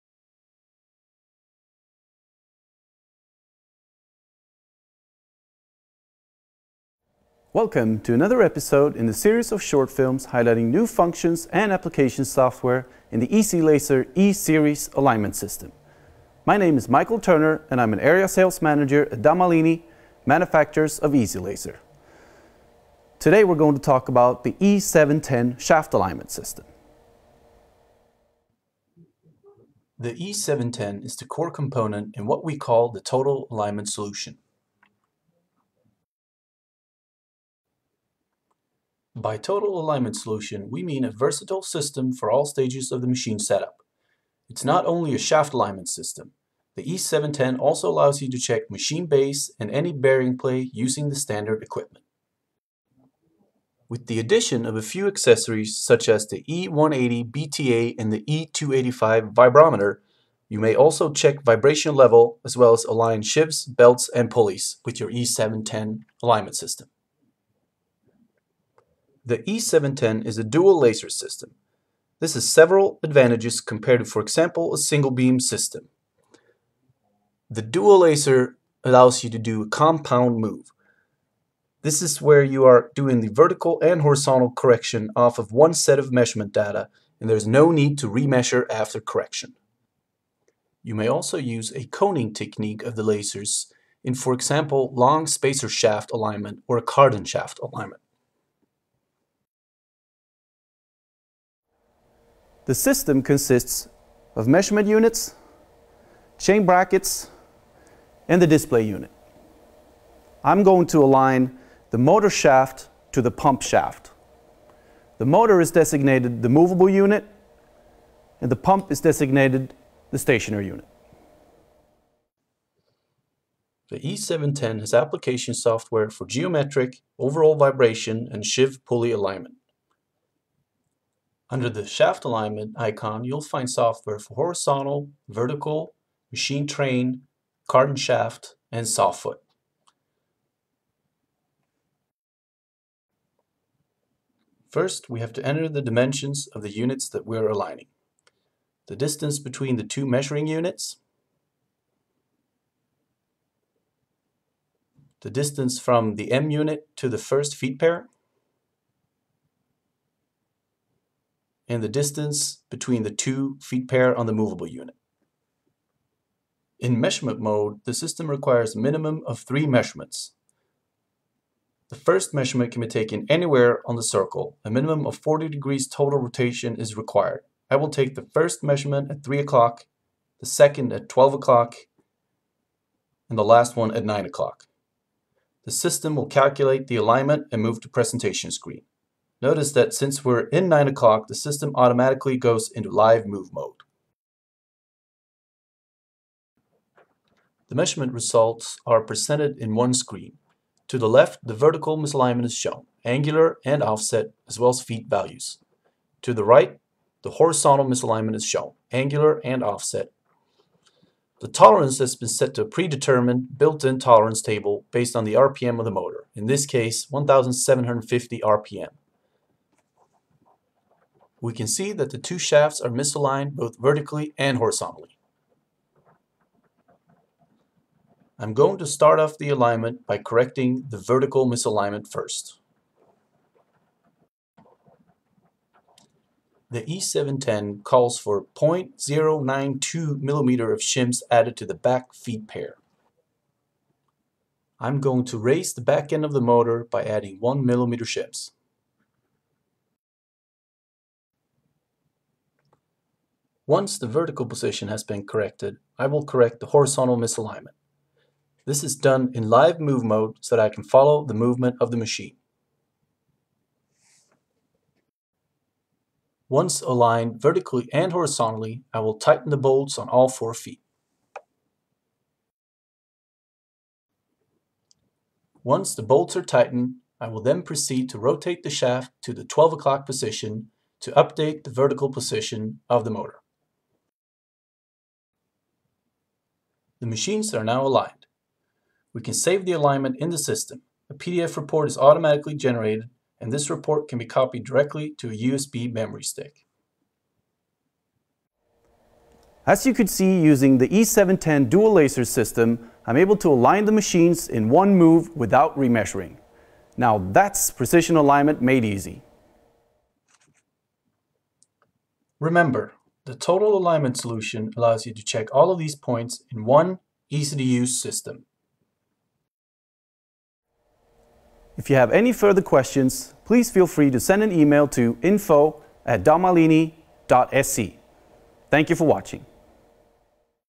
Horizontal alignment with Easy-Laser® E710, the Total Alignment Solution
Easy-Laser® E710 is the Total Alignment Solution. It's a measurement system for all important steps of machinery set-up, such as checking base flatness, bearing play, vibration level, soft foot and of course alignment. In this movie we show how easy the Horizontal machine alignment procedure is.